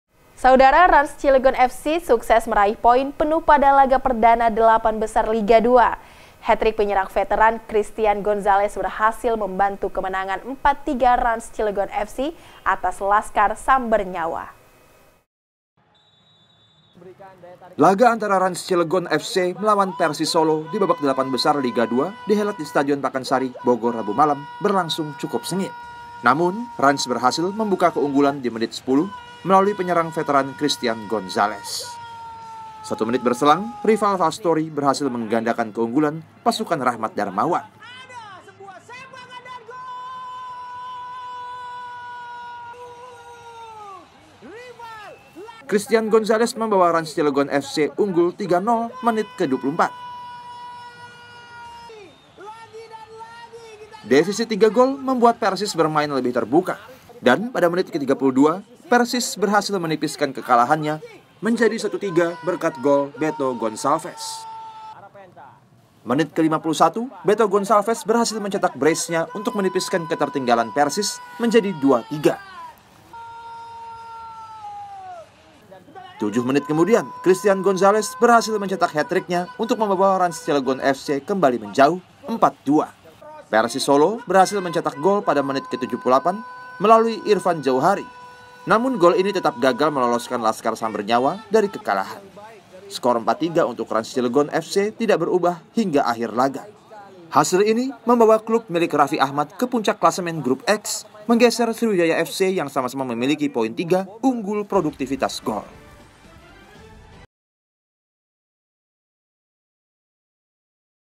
Rans Cilegon FC Raih Poin Penuh di Laga Perdana 8 Besar Liga 2
BOGOR, KOMPAS.TV - Rans Cilegon FC sukses meraih poin penuh pada laga perdana delapan besar liga 2.

Penyerang veteran, Christian Gonzales berhasil mencetak hattrick saat Rans Cilegon FC mengalahkan Persis Solo.

Sejak awal laga yang dihelat di Stadion Pakansari, Bogor berlangsung cukup sengit, namun Rans berhasil membuka keunggulan menit ke-10 melalui penyerang veteran, Cristian Gonzales.
Satu menit berselang, Rifal Lastori berhasil menggandakan keunggulan pasukan Rahmad Darmawan. Di menit ke-24 Christian Gonzales kembali membawa Rans Cilegon FC unggul 3-0.

Defisit 3 gol membuat Persis bermain lebih terbuka, dan pada menit 32, Persis berhasil menipiskan kekalahannya menjadi 1-3 berkat gol, Beto Goncalves.

Menit  ke-51, Beto Goncalves berhasil mencetak bracenya untuk menipiskan ketertinggalan Persis menjadi 2-3. Tujuh menit kemudian, Christian Gonzales berhasil mencetak hattricknya untuk membawa Rans Cilegon FC kembali menjauh 4-2.

Persis solo berhasil mencetak gol pada menit ke-78, melalui Irfan Jauhari, namun gol ini tetap gagal meloloskan laskar samber nyawa dari kekalahan. (*)